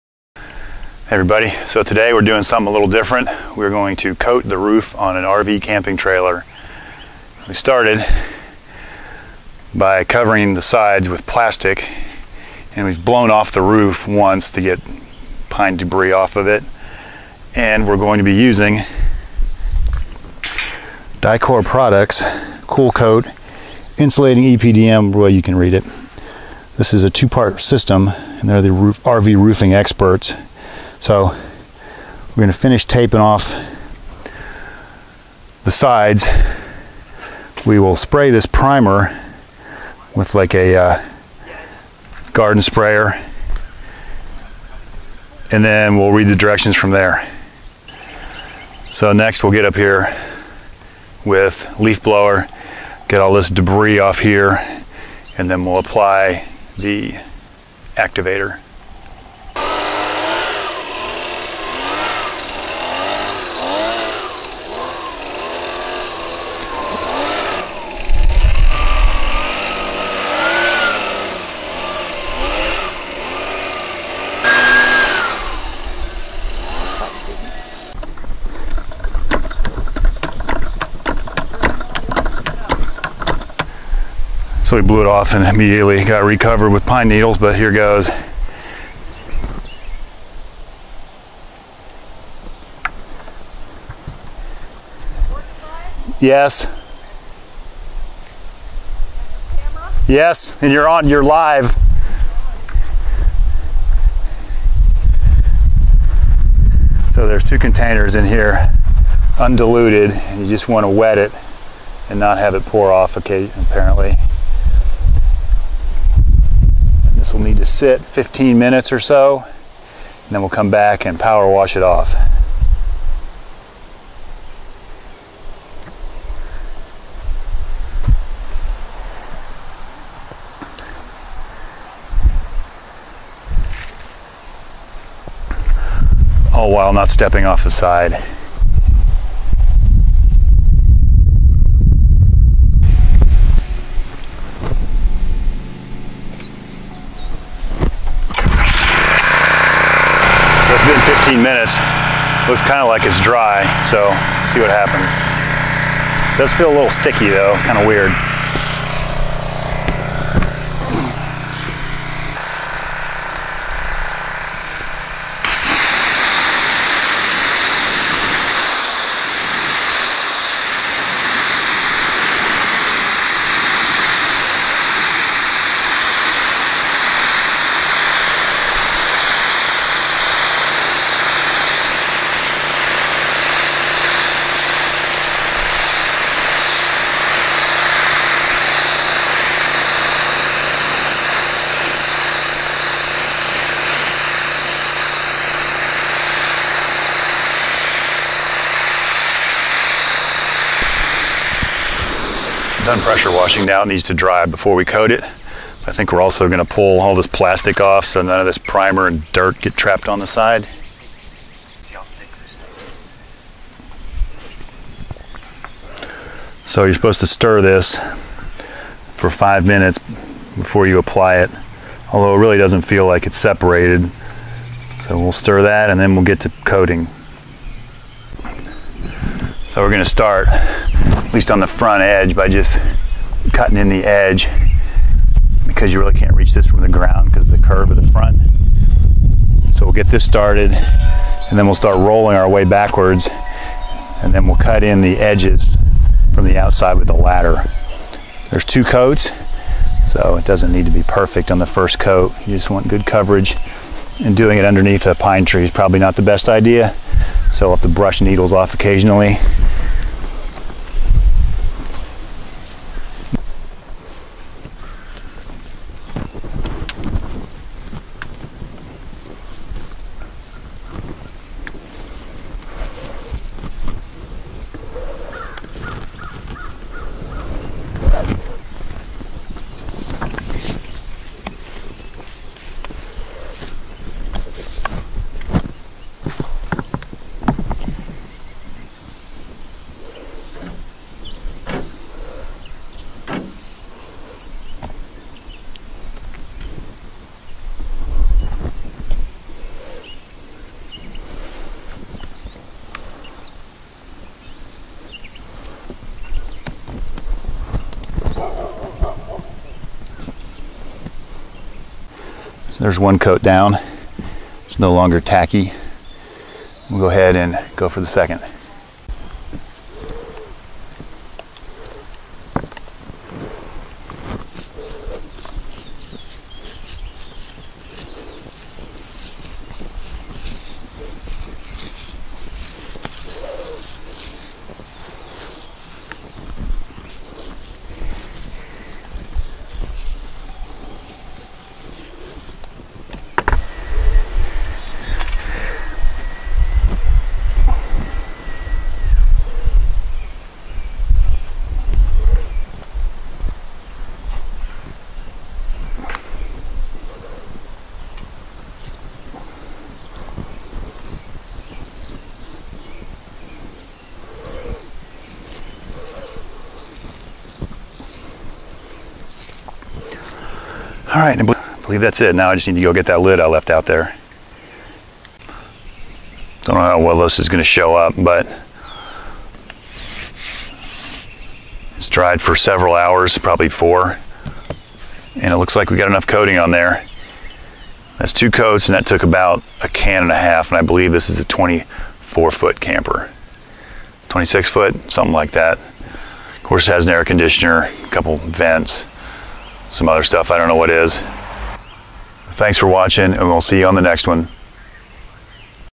Coating the Roof of an RV Camping Trailer
In this video we apply a roof coating to an RV camping trailer. The roof had no visible problems so this was done more so for preventative maintenance. The product is Dicor Cool Coat. The trailer is considered a 28 foot model and we ended up using about 1.5 cans in 2 heavy coats.